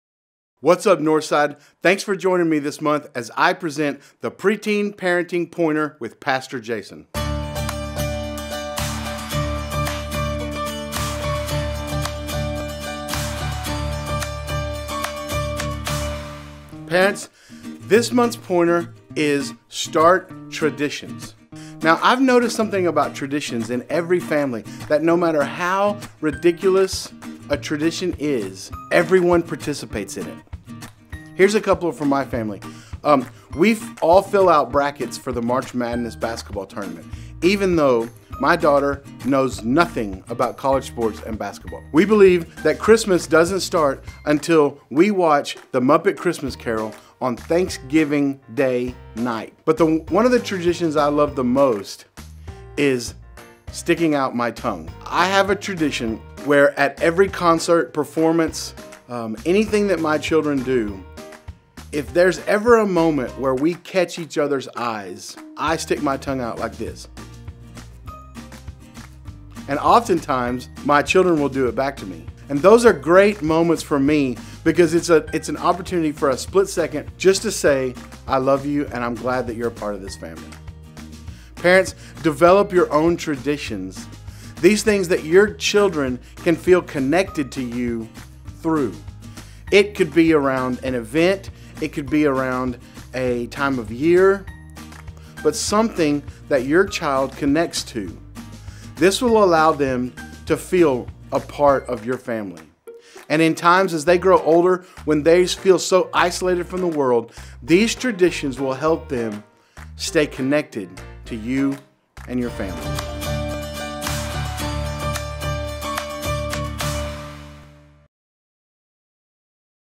PreTeen Parent Pointer with Pastor Jason: April 2022
Why am I talking about sticking out my tongue? To connect with my kids, of course!! Watch to learn what family traditions have to do with being a parent.